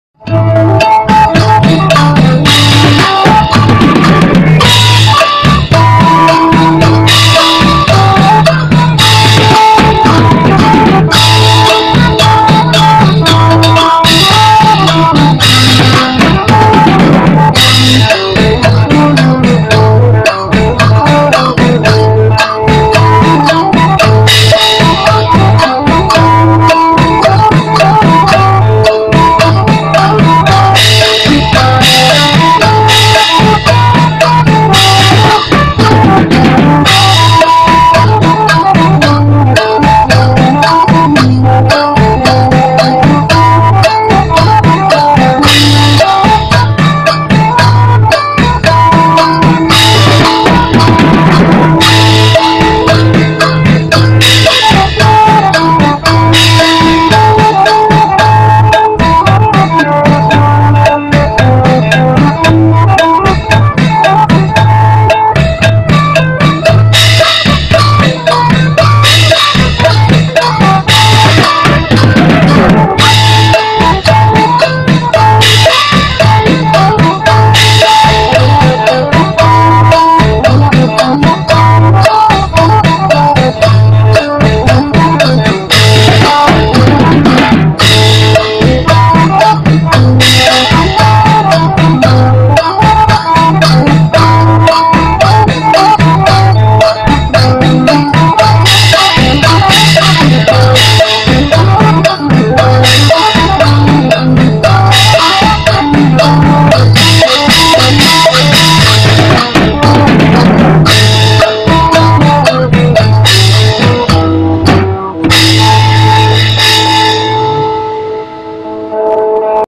ຈັງຫວະລຳເຕີ້ຍໄວໆ จังหวะลำเตยไวๆ
ເສບທີ່ງານຜ້າປ່າບ້ານສອງຫ້ອງ ເມືອງ ຊົນບູລີ